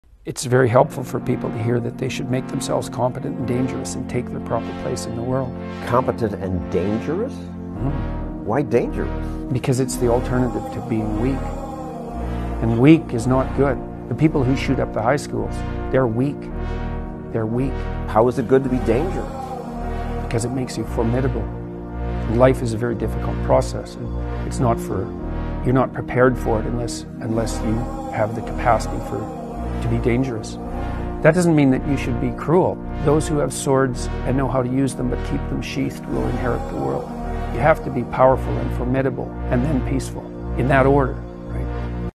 Make yourself competent and dangerous – Jordan Peterson
Spoken by Jordan Peterson